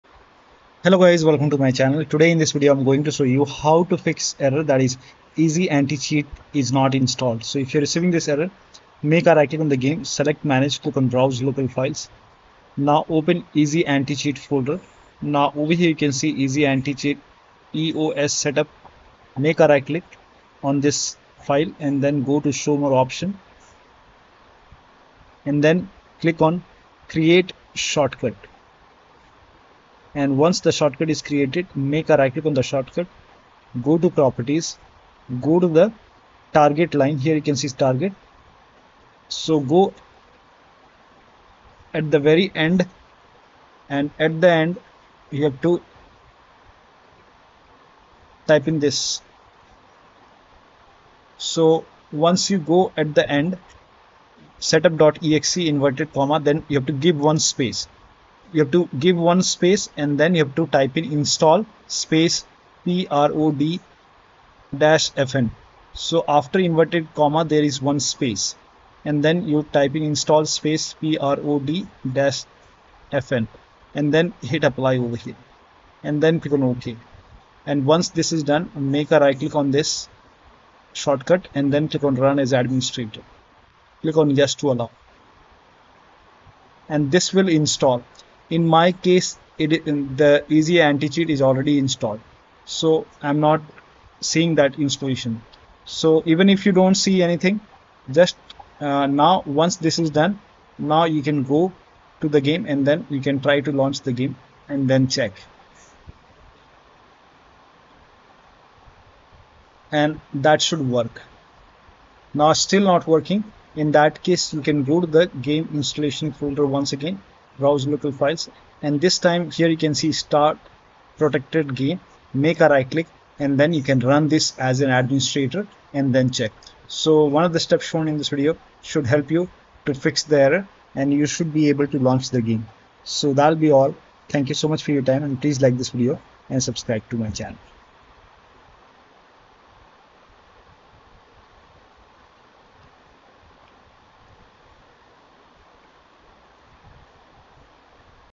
Fix Killing Floor 3 EasyAntiCheat Error EasyAntiCheat Is Not Installed On PC
How To Fix Killing Floor 3 EasyAntiCheat Error EasyAntiCheat Is Not Installed On PC
install prod-fn